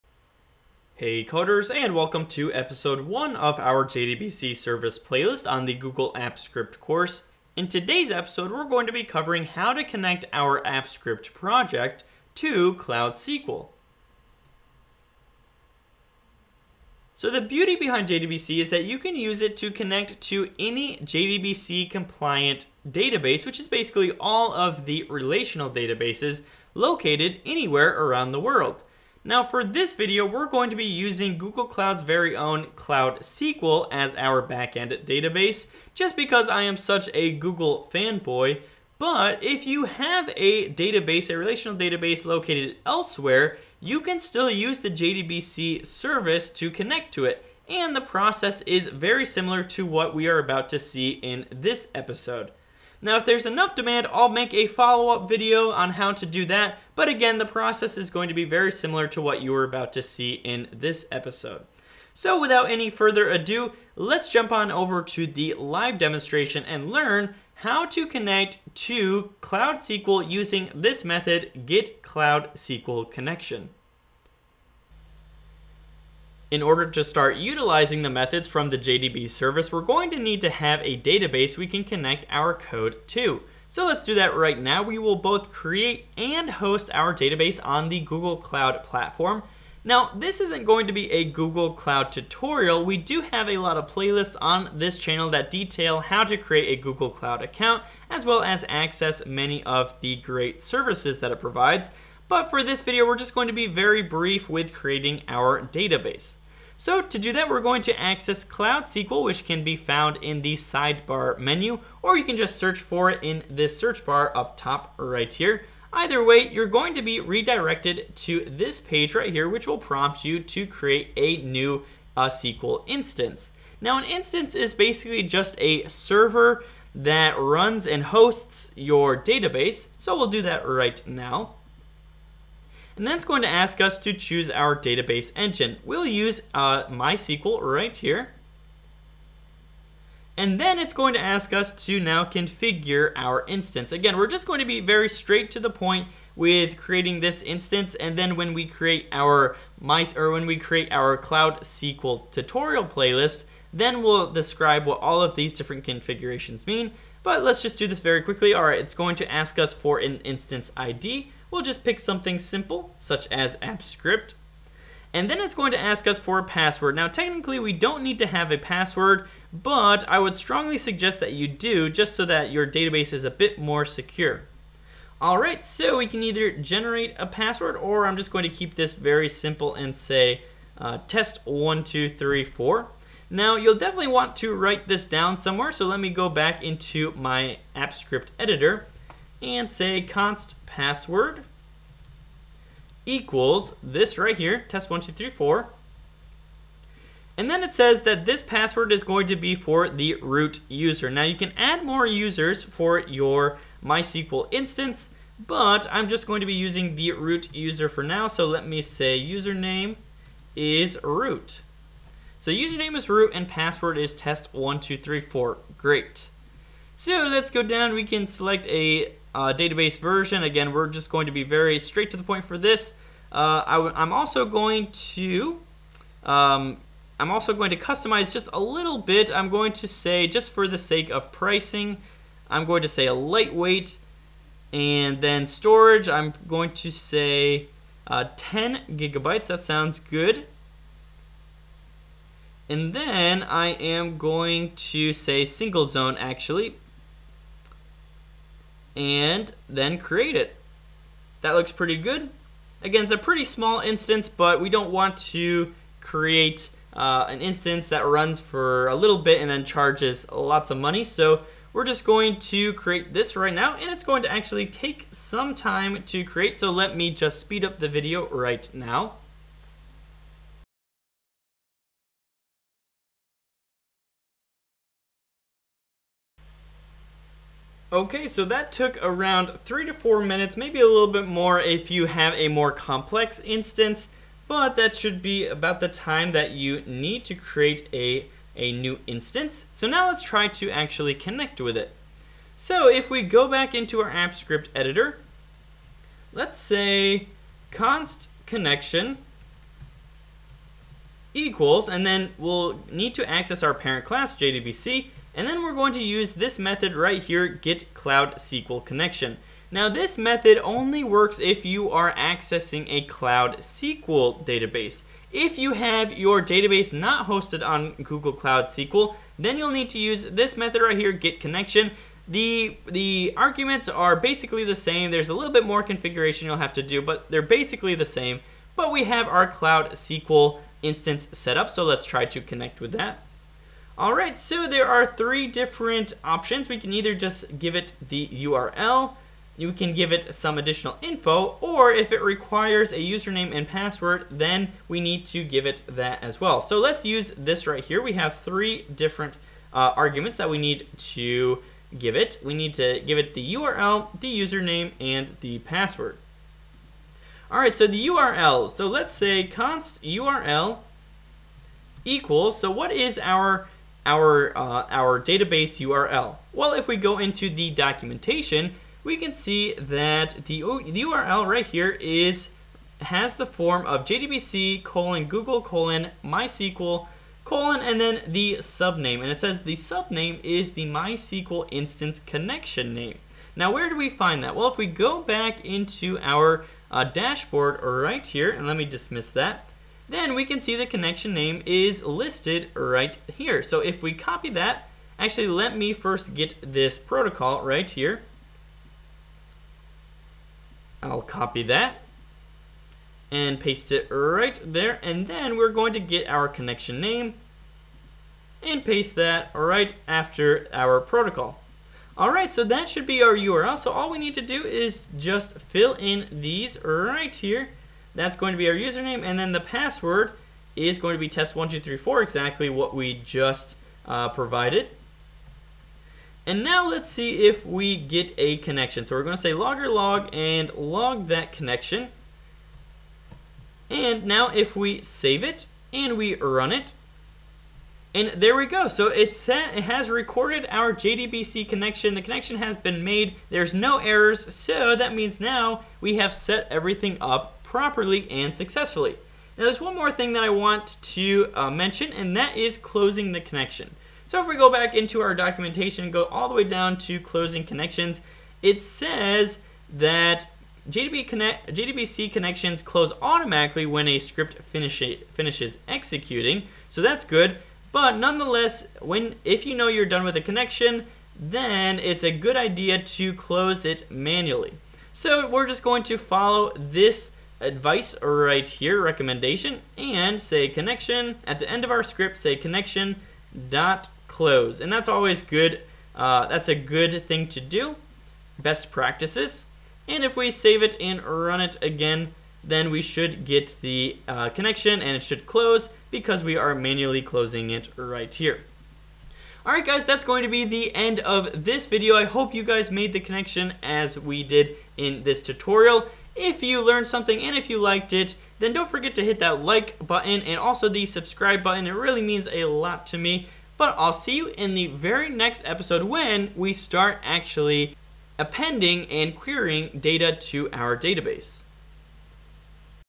Connecting to Cloud SQL - Apps Script | JDBC Service ~ Episode 16.1
Hey Coders! Do you store your data in Cloud SQL? Great choice! But did you know you can access that data from Apps Script in just one simple line of code? Using the JDBC Service, you can read and write data to your Cloud SQL instances. Before you start performing operations on it, however, you'll need to know how to make the connection. This video will show you how!

❤️ Support the ongoing growth of this channel, get exclusive perks, and receive consultation on your projects!

🕒 Timestamps
0:00 Intro
1:15 Creating a Cloud SQL Database
4:37 Connecting to Cloud SQL via getCloudSqlConnection()
5:57 Defining our Cloud SQL Database URL
7:06 Testing the Connection
7:29 Best Practice: Closing JDBC Connections